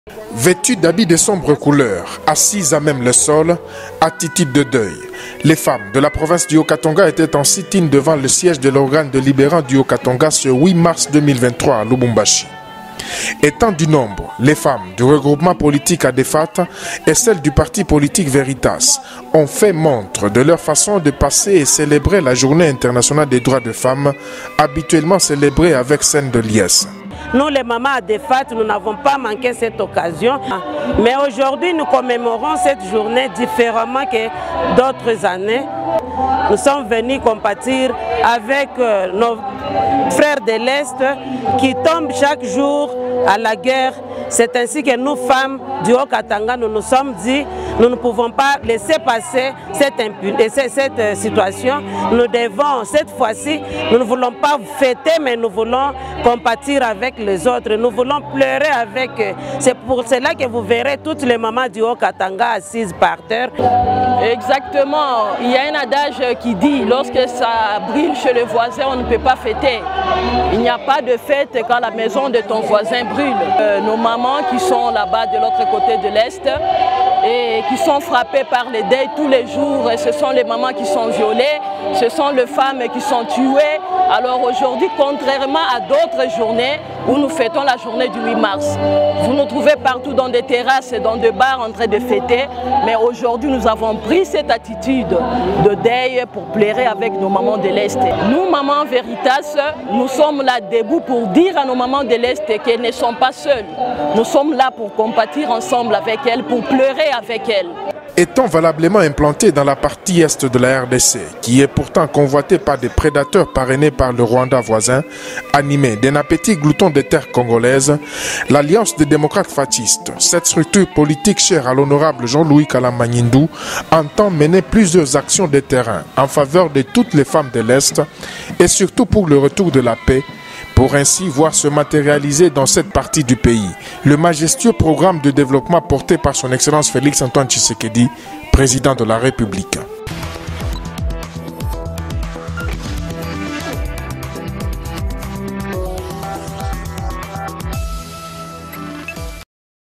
RDC - JOURNEE INTERNATIONALE DE LA FEMME 2023: LES MAMANS ADEFAT et VERITAS EN SIT-IN A LUBUMBASHI
En marge de la journée internationale des droits des femmes, les mamans de l’Alliance des Démocrates Fatshistes, A.DE.FAT Regroupement politique et  du parti politique VERITAS – RDC, étaient en sit-in ce 08 Mars 2023 devant le siège de l’Assemblée Provinciale à Lubumbashi.